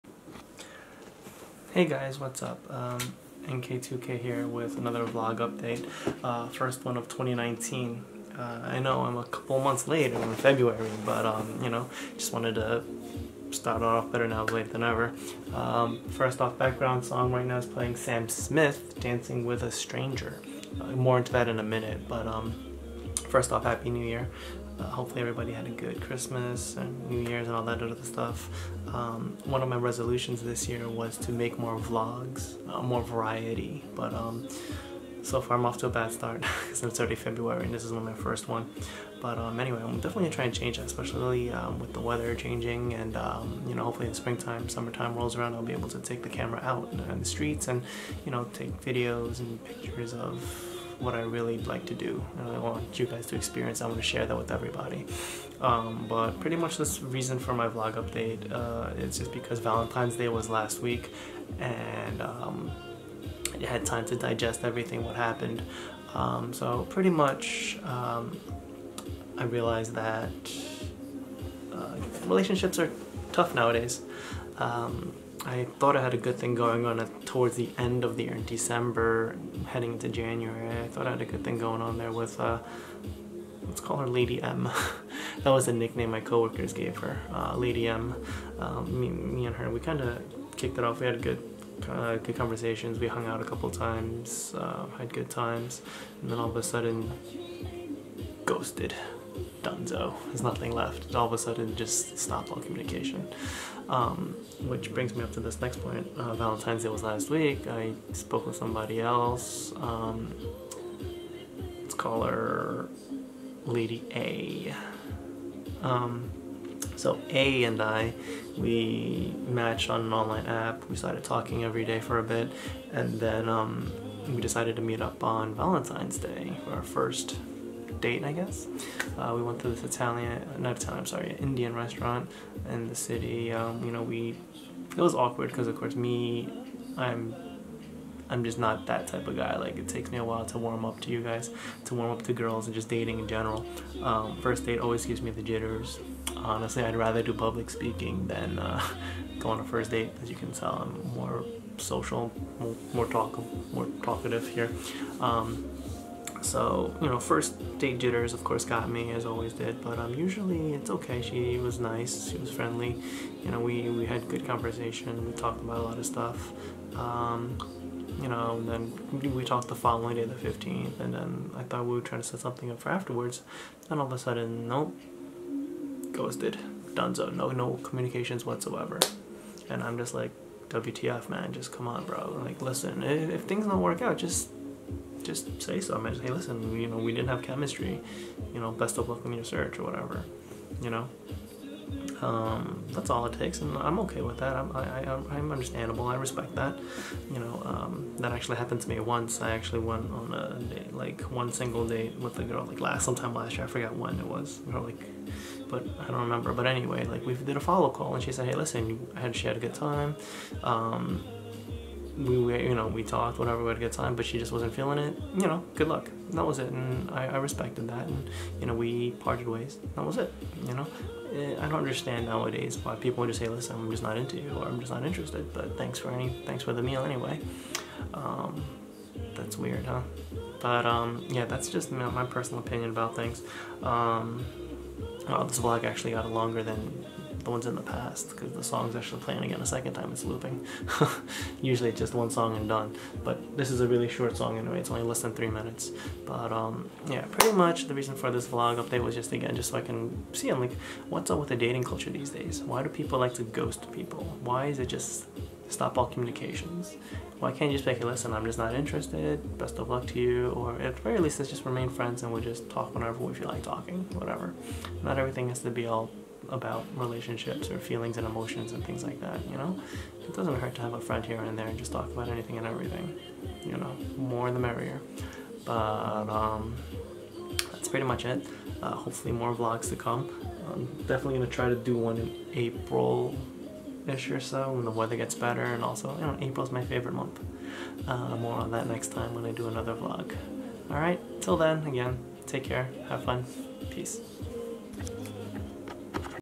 VLog # 13!! First of 2019. Ghosts....
First Vlog of 2019!! Are you guys afraid of ghosts? How about your thoughts on being ghosted? Here's my thoughts amongst other things.